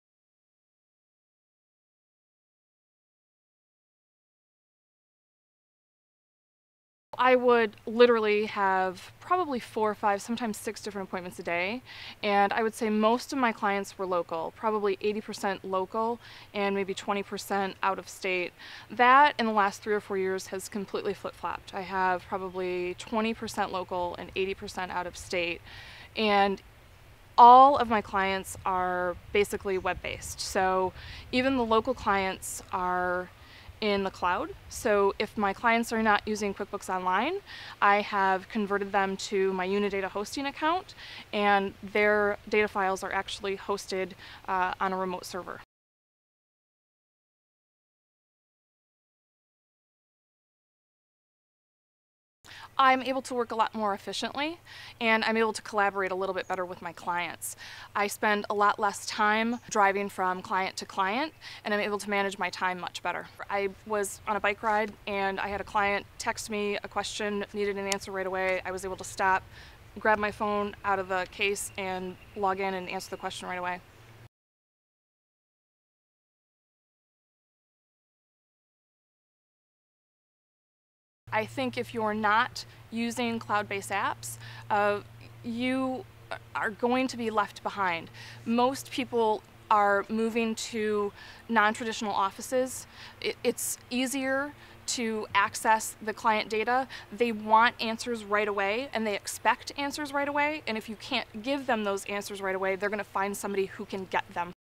Stacy Kildal: Her Practice Before and After the Cloud
QuickBooks expert Stacy Kildal of Radio Free Quickbooks has experienced a lot of great payoffs since moving her accounting practice to the cloud. Now, she can work more efficiently and can better manage her time, with clients from around the country. Her advice to accountants? Learn about and leverage the cloud, or you're going to get left behind!